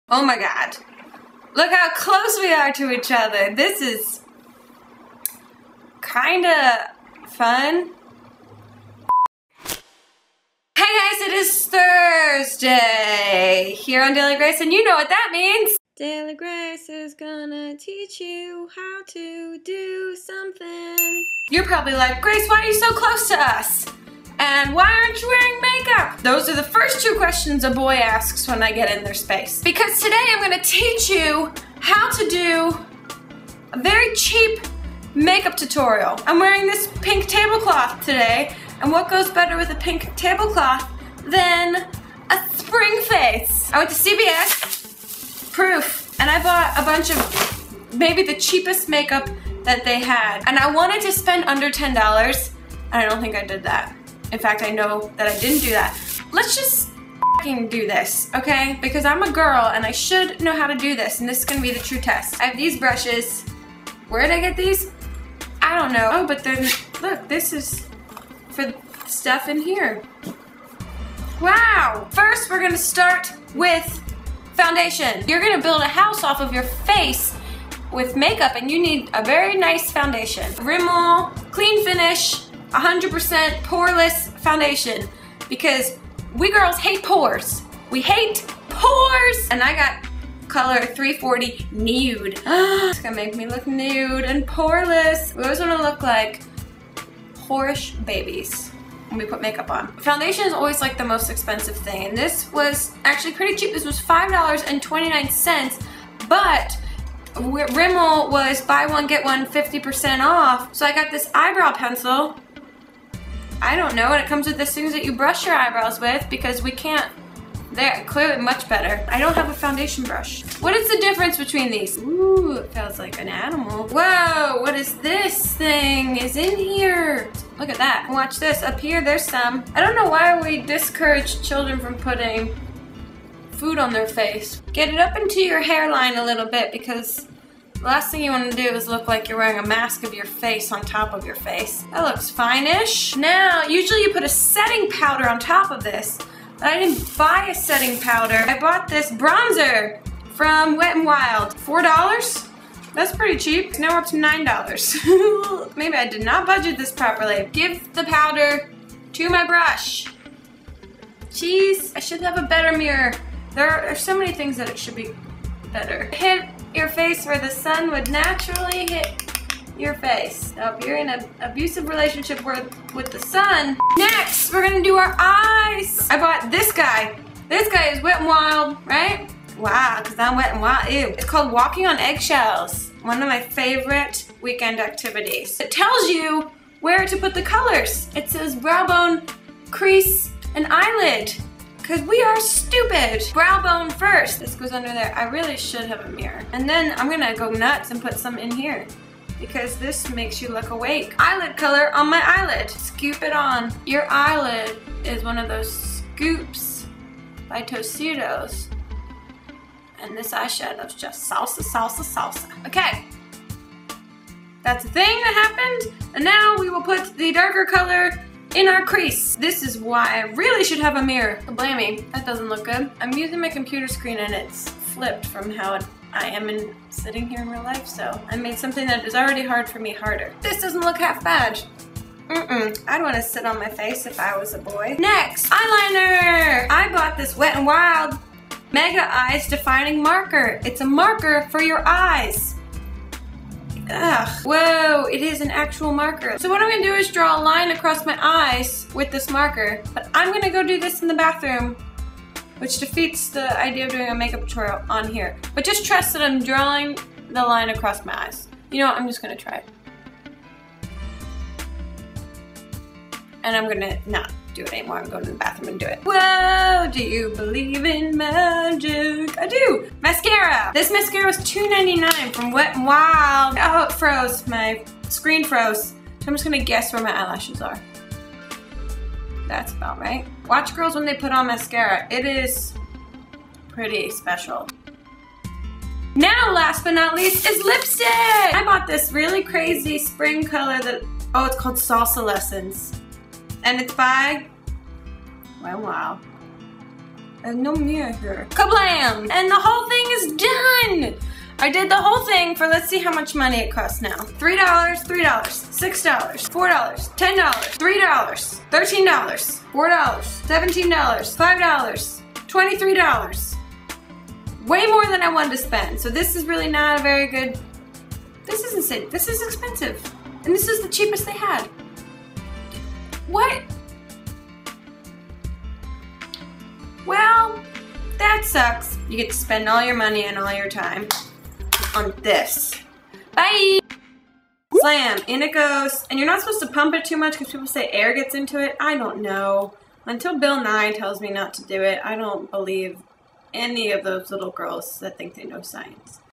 CHEAP SPRING MAKEUP TUTORIAL
DailyGrace gives you a cheap makeup tutorial using all drugstore products!